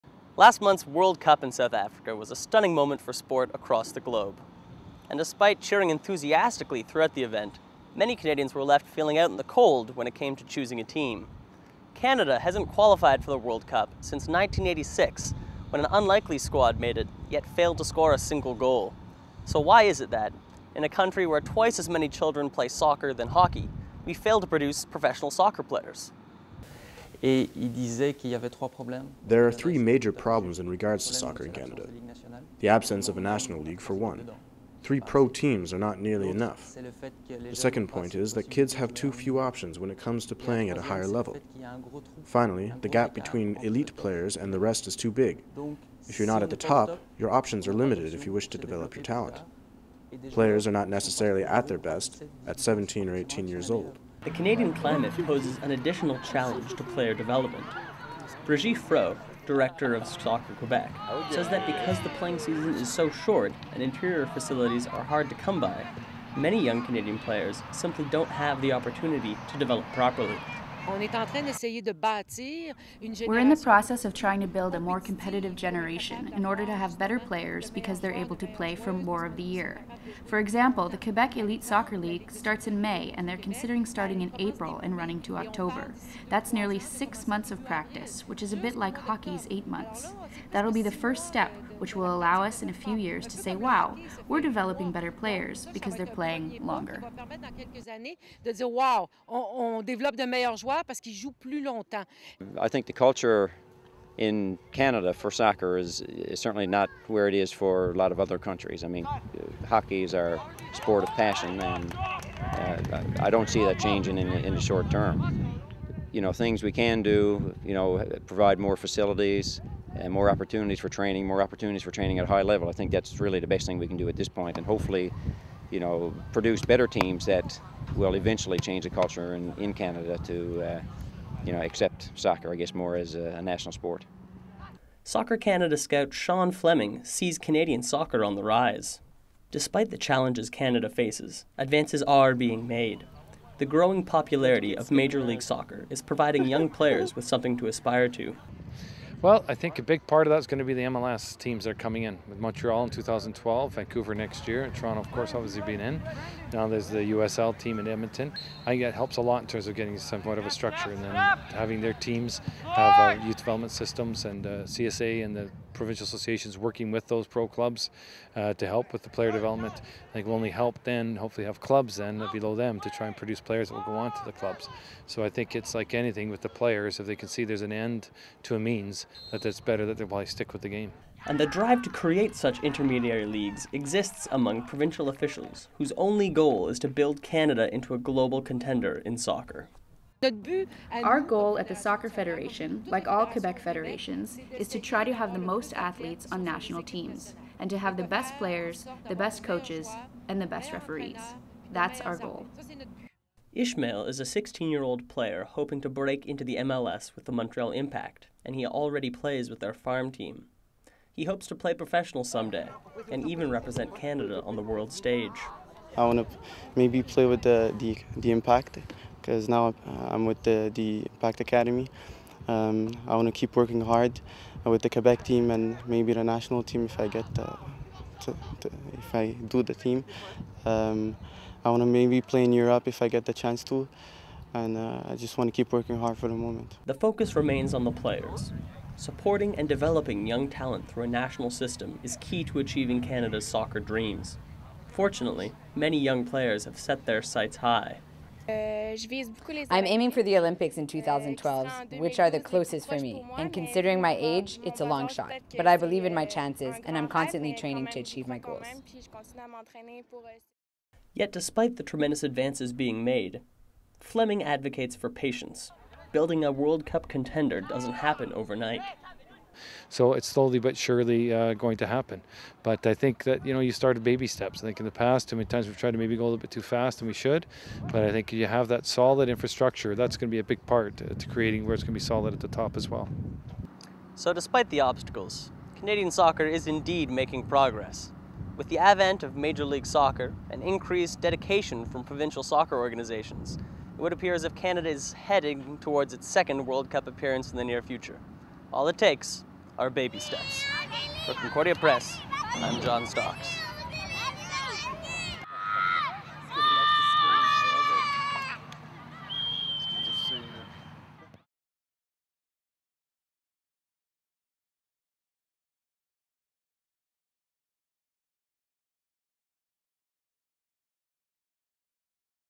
The State of Youth Soccer in Quebec documentary
Canada is still a junior on the world soccer stage, but progress is being made. John Stocks looks deeper into the soccer roots being laid in Quebec. July, 2011.